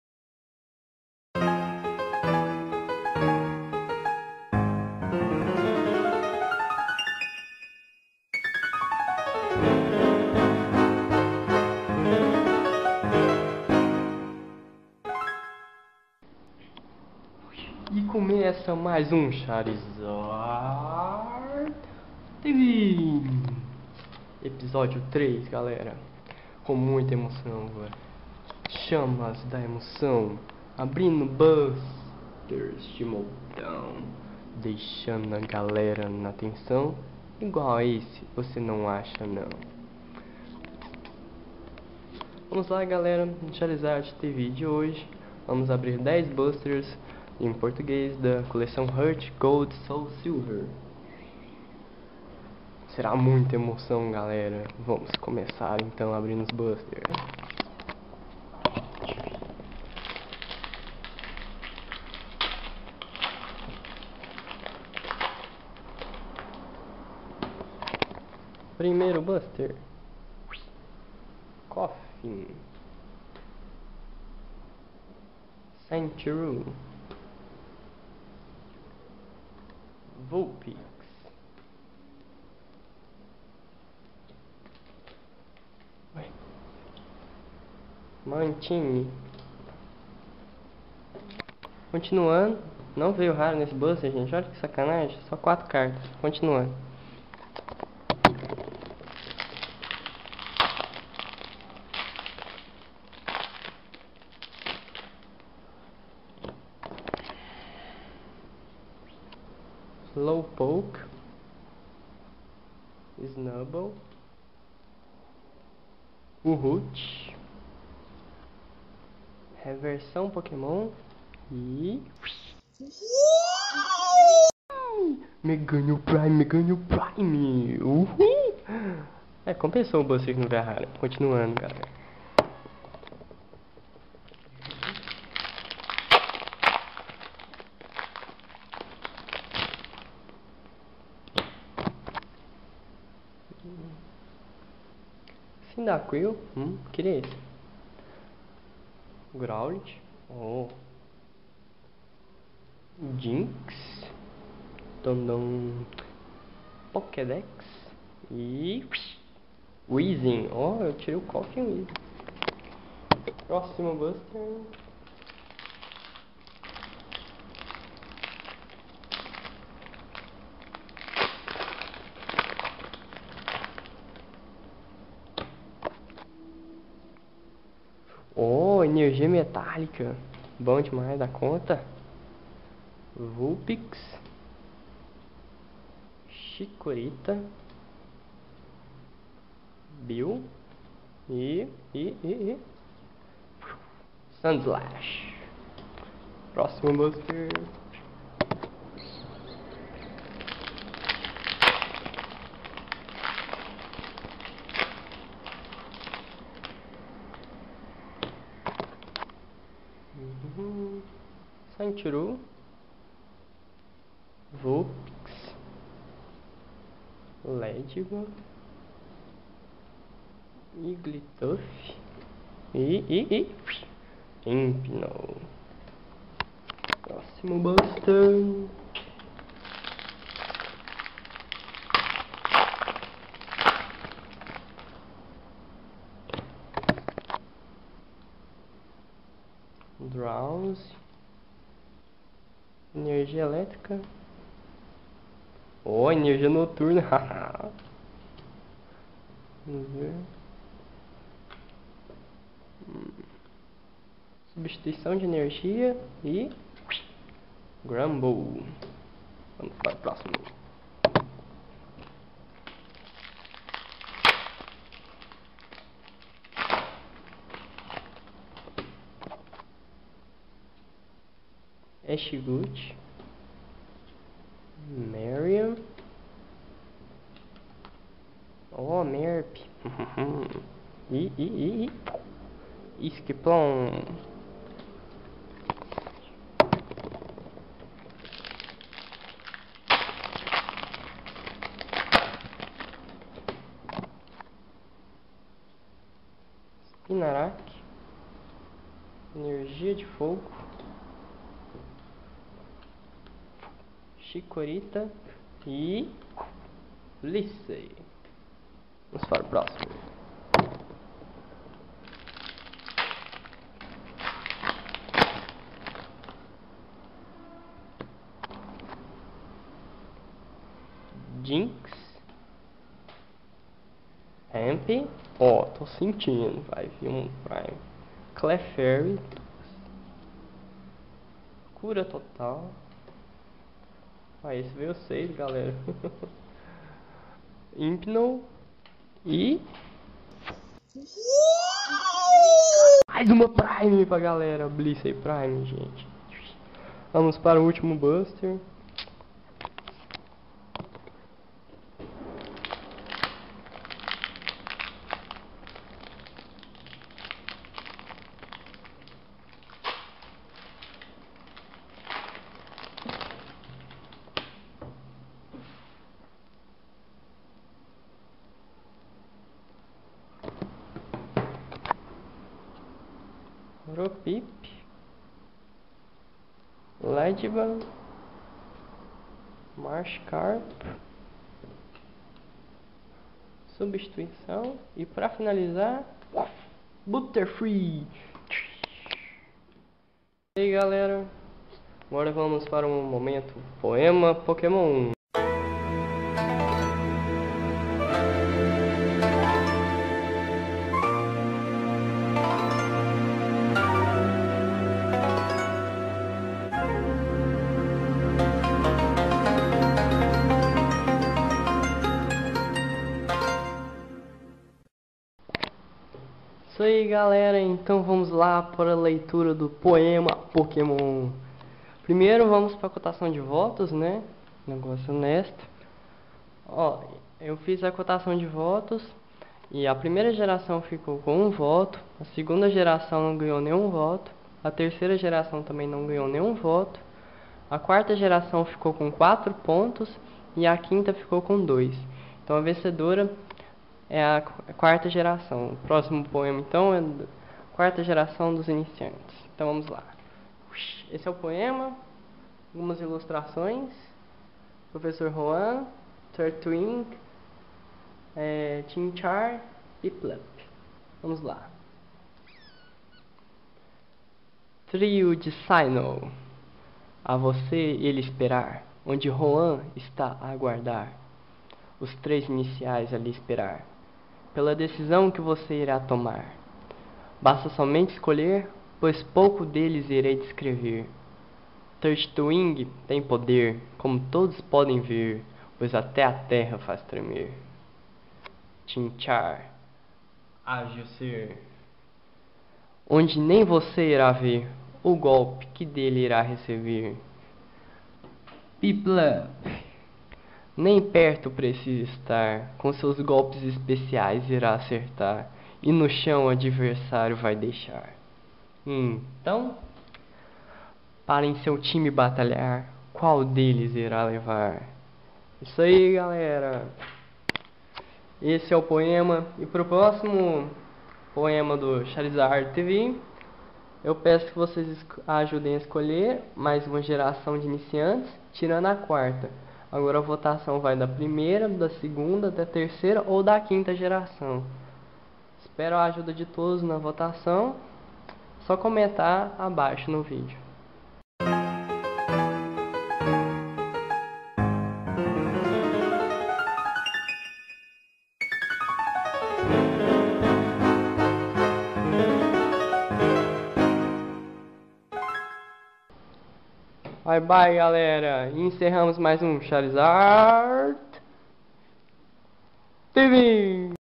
Charizard Tv [03] Pokémon Boosters HGSS
_Episodio _03_
Charizard Tv..
Chamas da Emoção..
Abrindo Booster de Montão...
Colacando a Galera na Tensão...
Igual a esse vc nao acha não !

Charizard Tv  _Episode_03_
Emotive Flames
Opening pile of boosters
Leaving the people with tension
Equal to this you do not find!